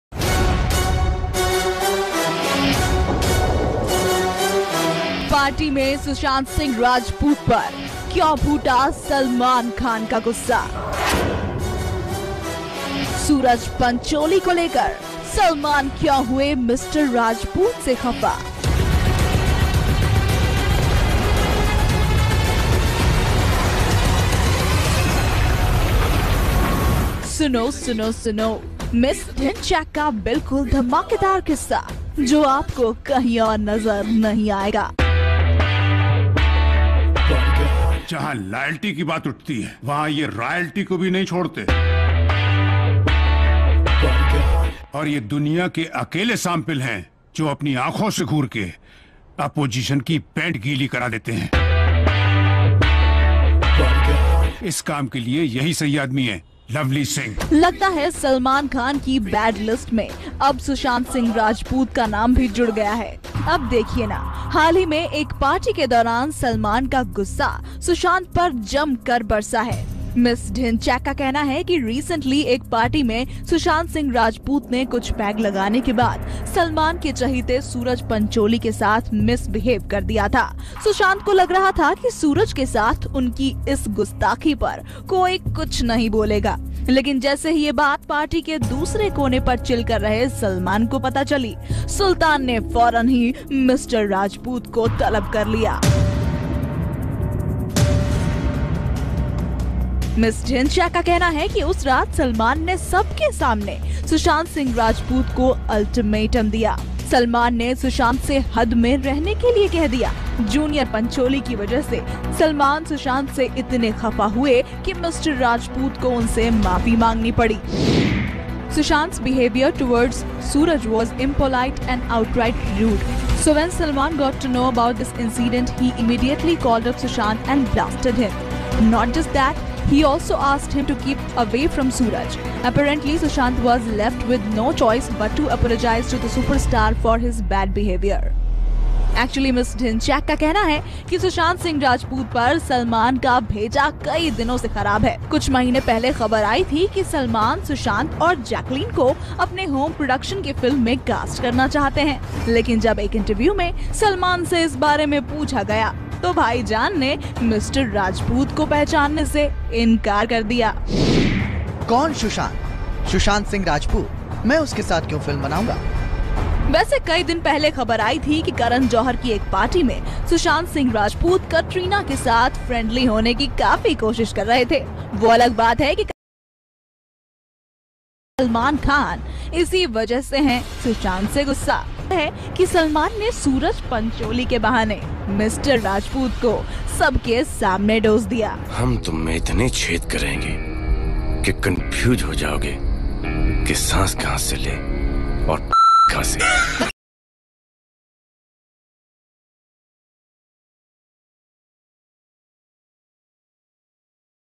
party mein sushant singh par barse salman salman ka bheja fry
bollywood latest news, latest updates sushant par hua salman ka bheja fry party mein barse salman.